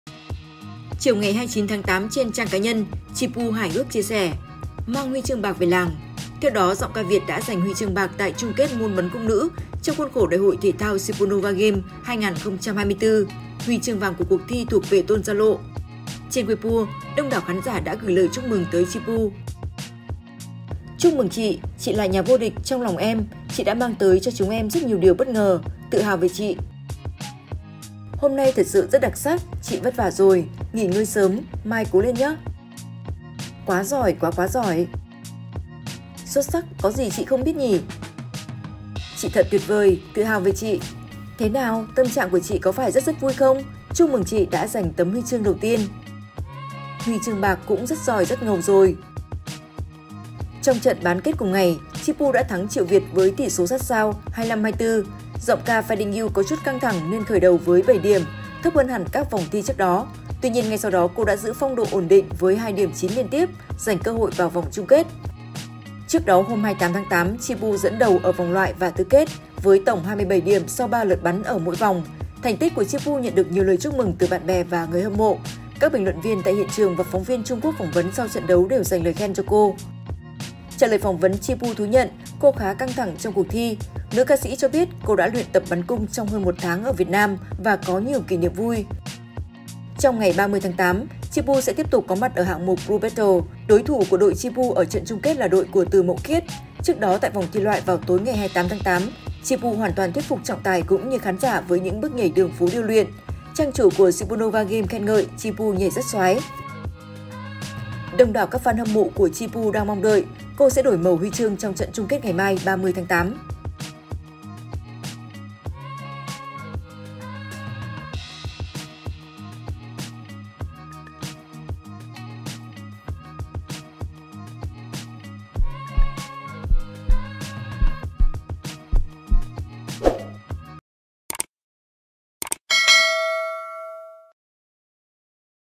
Super Novae Games: Chi Pu giành HCB bắn cung, vẫn còn cơ hội đổi màu huy chương ở ngày thi đấu thứ 3
Ngày 28/8/2024, l Chi Pu tham gia 2 hạng mục là bắn cung và crew battle trong khuôn khổ cuộc thi Super Novae Games 5.
Cảm ơn các bạn đã ghé thăm Dưa Showbiz.